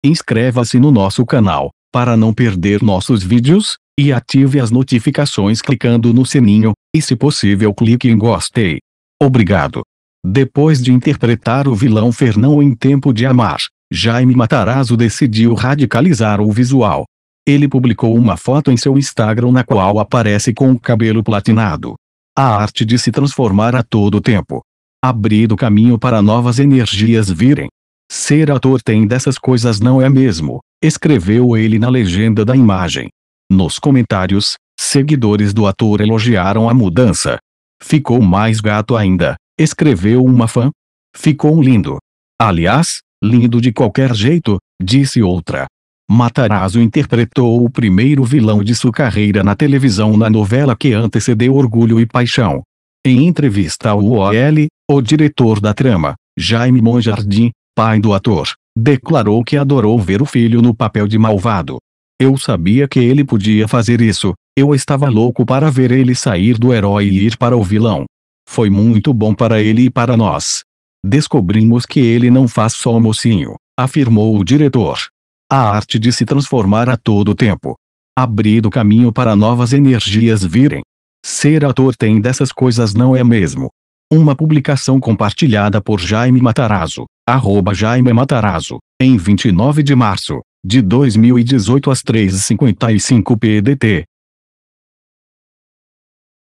Jayme Matarazzo radicaliza visual e surge com o cabelo platinado 30.03.2018 09h10
Depois de interpretar o vilão Fernão em "Tempo de Amar", Jayme Matarazzo decidiu radicalizar o visual. Ele publicou uma foto em seu Instagram na qual aparece com o cabelo platinado. "A arte de se transformar a todo tempo. Abrindo caminho para novas energias virem. Ser ator tem dessas coisas não é mesmo", escreveu ele na legenda da imagem. Nos comentários, seguidores do ator elogiaram a mudança. "Ficou mais gato ainda", escreveu uma fã. "Ficou lindo! Aliás, lindo de qualquer jeito", disse outra. ...